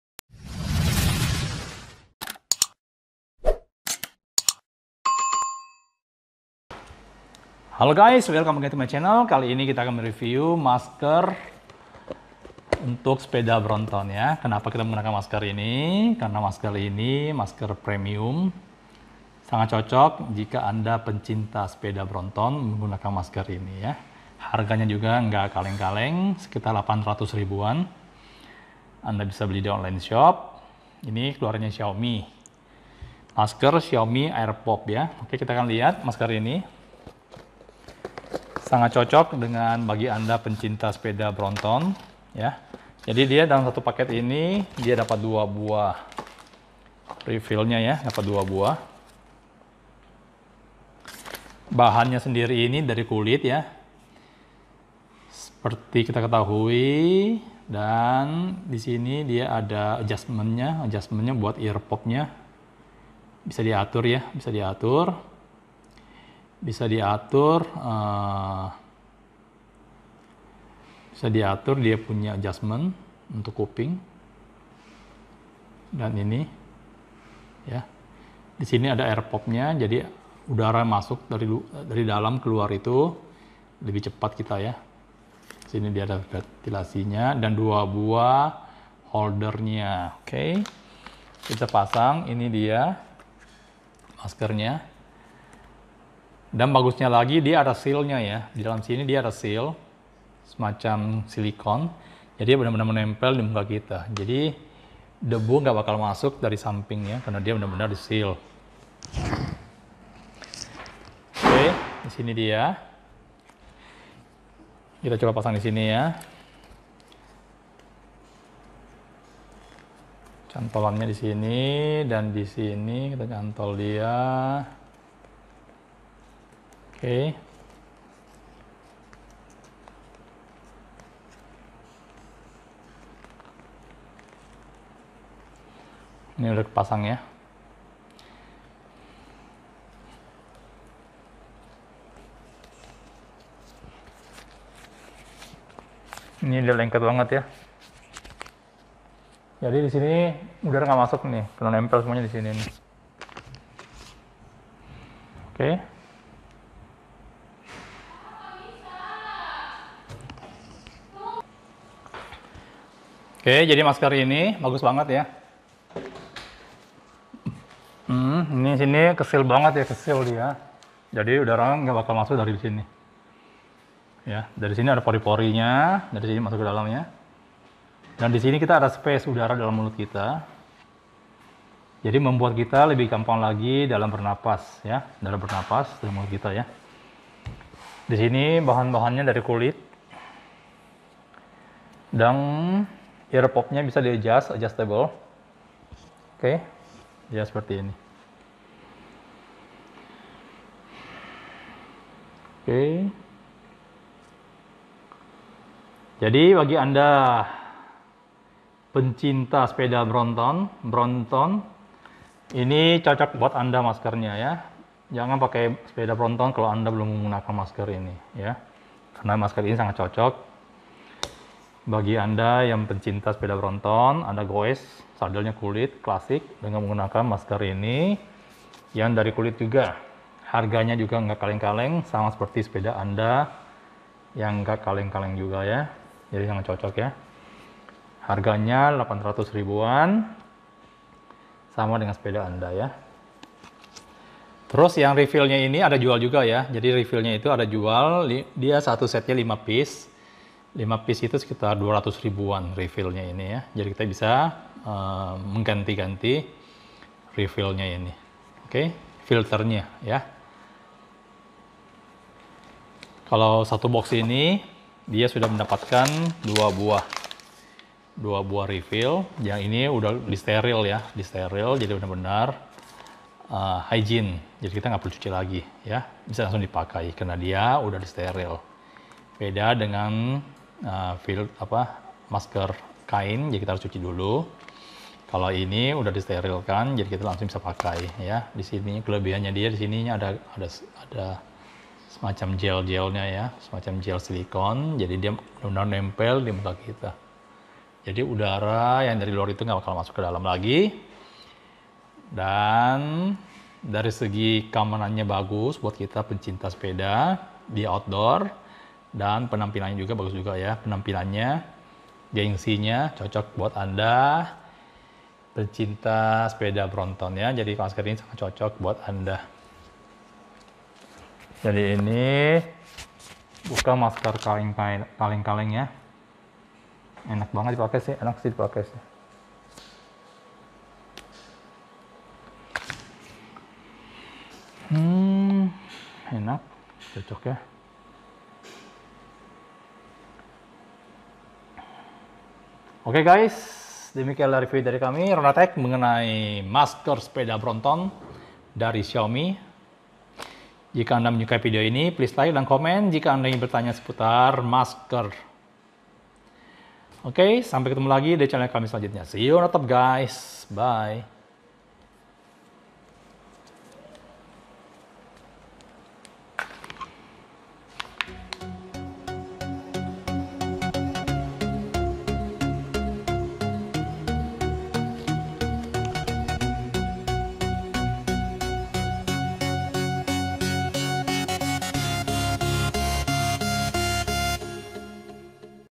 Masker Untuk Penguna Sepeda Bronton
Designed for Air Pollution Efisiensi perlindungan dari masker anti-kabut konvensional berbanding terbalik dengan permeabilitas udara, dan masker dengan efek perlindungan yang kuat sebagian besar tidak nyaman. Airwear dilengkapi dengan masker anti-asap. Teknologi sentuhan ringan digunakan bersama dengan teknologi Light Touch. Dengan kapas filter penangkap elektrostatik canggih, USG stereo 3D digunakan untuk menggabungkan perlindungan dan kenyamanan yang sangat efisien. Dalam perlindungan partikel polusi minyak dan non-minyak, dapat mempertahankan permeabilitas udara yang baik. Masker dapat merespon berbagai penyebab polusi udara. 360° Touch Sealed Seamlessly Filter ini sesuai dengan cincin penyegelan yang dapat diserap anti-kabut, yang berarti dapat disegel ke segala arah dengan sentuhan, dan tekanan dapat tersebar tanpa tanda. Perlindungan lapis ganda dari ujung buntu di kedua sisi jembatan hidung, dagu, dll. Memungkinkan masker untuk menyesuaikan dengan kontur wajah, dan perlindungan intim kadang-kadang tidak dirasakan